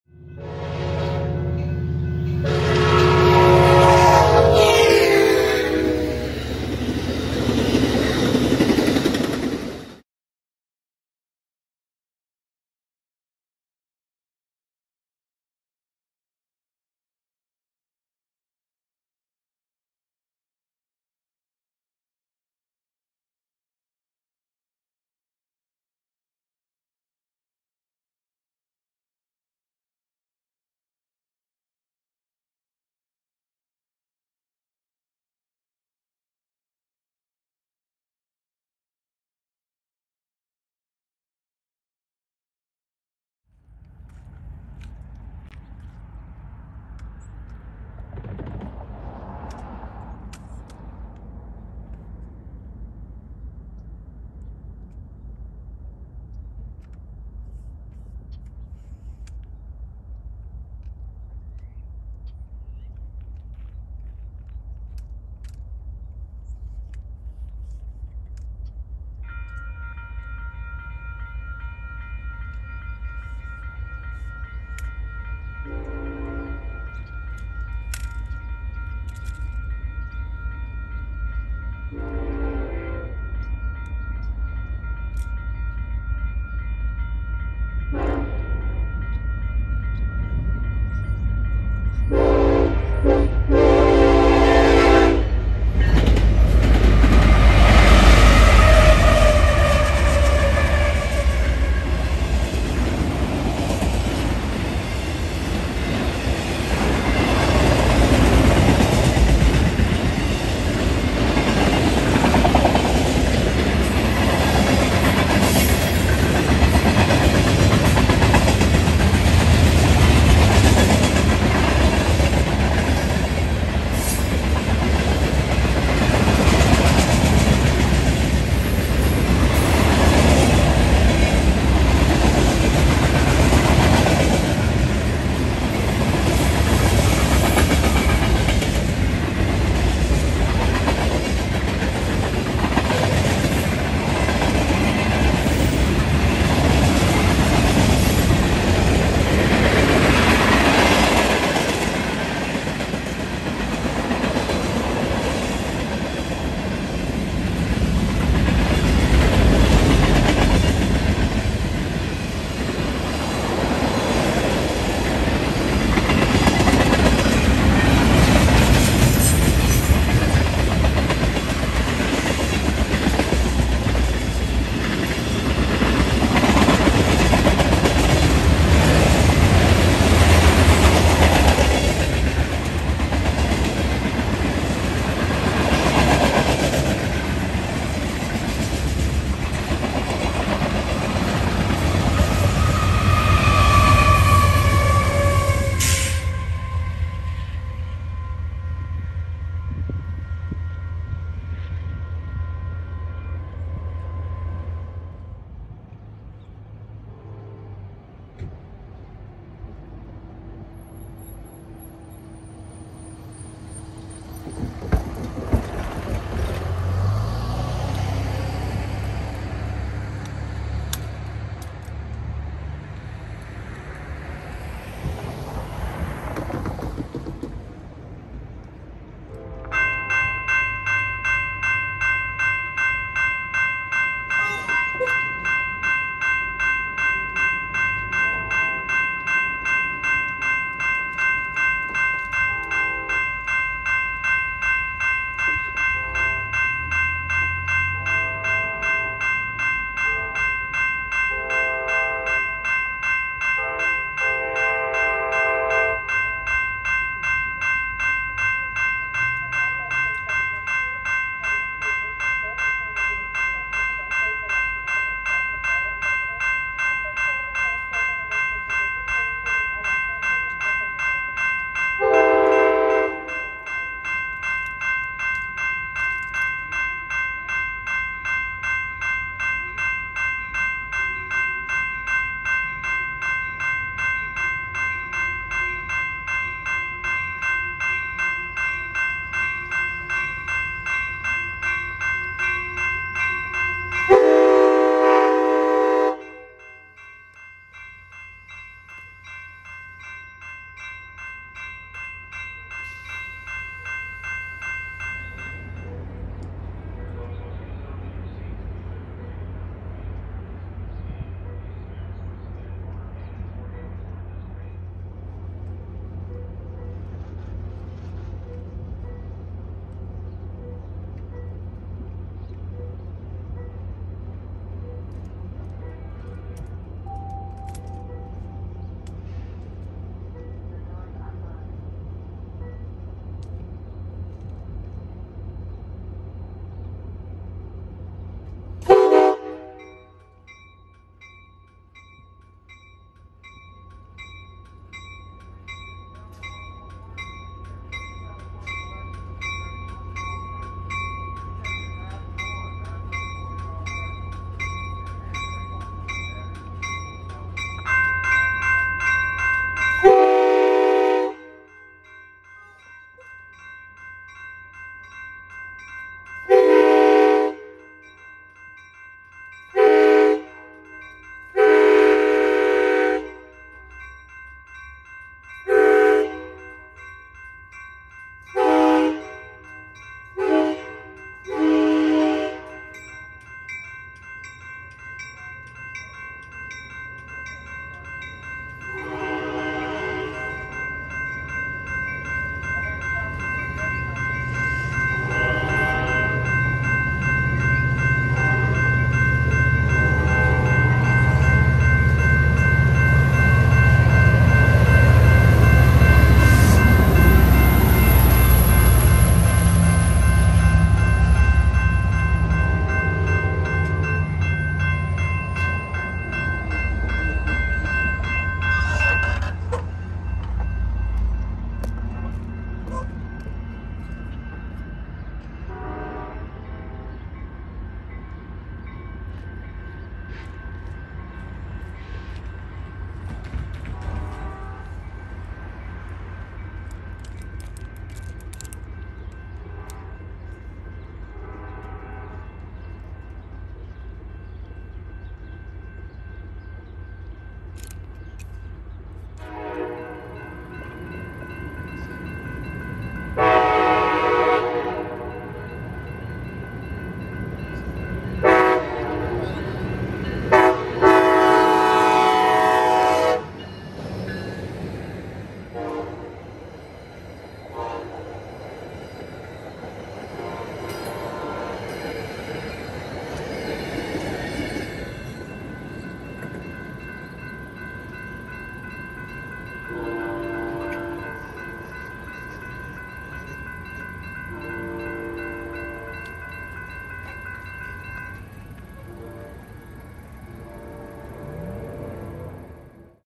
The First Trains of 2022 in Lodi and Stockton, CA!
-1/1/2022-
Wow, how fast time has went! From one day to the next, we enter a new year. Sadly, the pandemic still rages on, but at least we still continue on. Expect more content this year, and I look forward to hopefully a good 2022! Stay safe everyone!

UP SD70ACe 8754 SB Manifest, E Armstrong Rd Crossing, Armstrong/Lodi, CA:
UP SD70ACe 8754, RL K5LLA
UP C44ACCTE 5727
UP C44ACM 7238
UP ET44AH 2717 DPU
UP C44ACM 7142 DPU

Amtrak 712, S San Joaquin St Crossing, Stockton, CA:
Amtrak CDTX Cabcar 6961, Great K5LA!
Amtrak CDTX F59PHI 2001

Amtrak 713, S Commerce St Crossing, Stockton, CA:
Amtrak CDTX SC-44 2101, RL K5LA
Amtrak CDTX NPCU 90218

                Nikon D3300